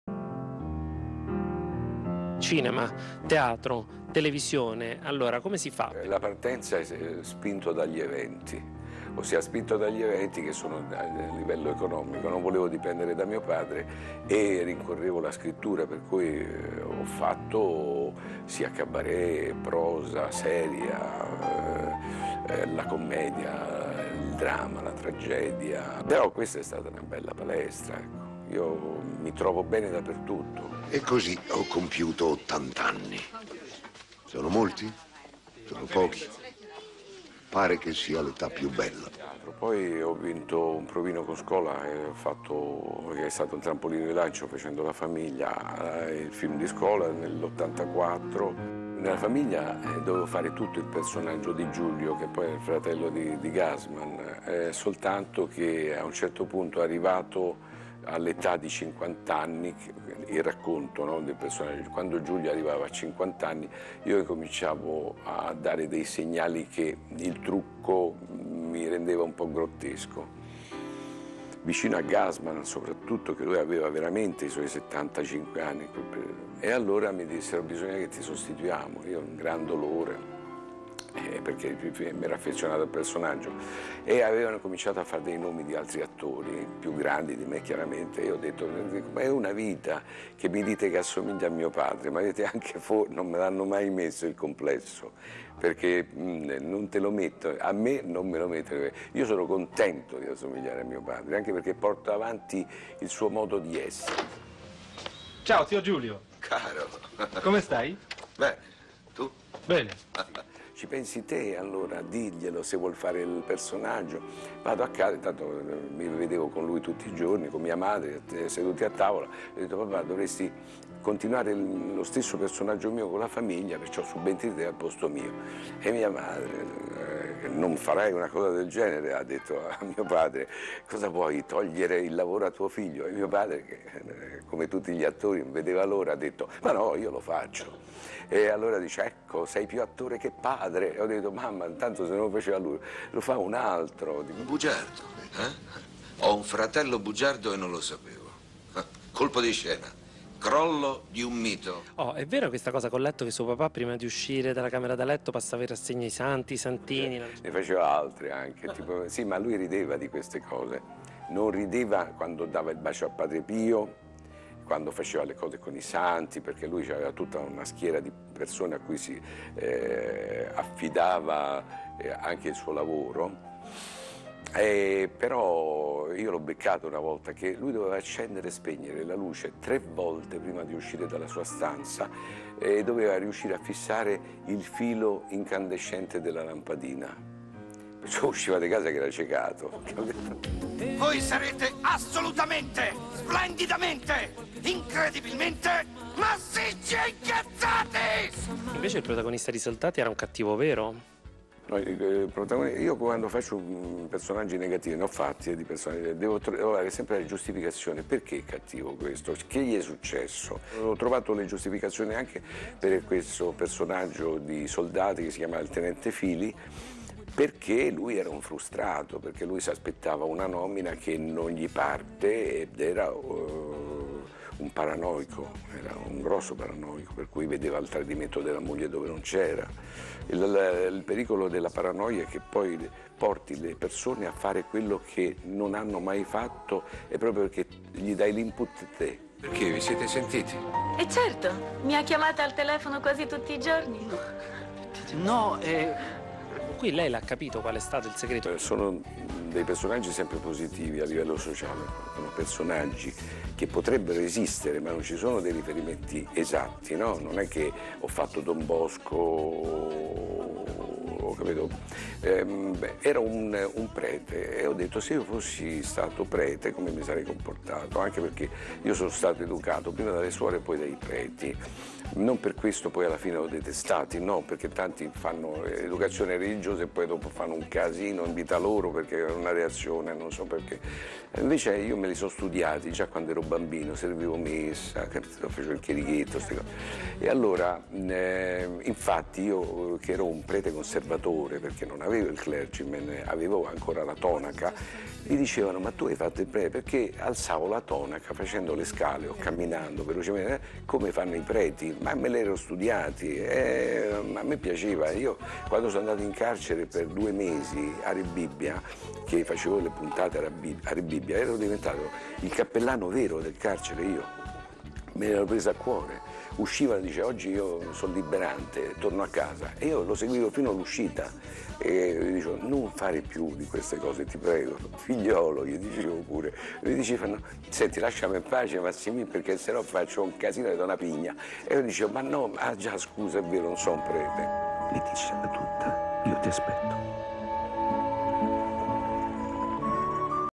Massimo Dapporto, attore a 360 gradi tra Cinema, Teatro e Tv
Massimo Dapporto racconta a Fabio Falzone il suo impegno di attore a 360 gradi tra teatro, tv e cinema. Alle telecamere di Effetto Notte, Dapporto svela aspetti inediti del rapporto con il papà Carlo e l'esperienza nel ruolo di sacerdote don Marco nella serie tv "Casa famiglia".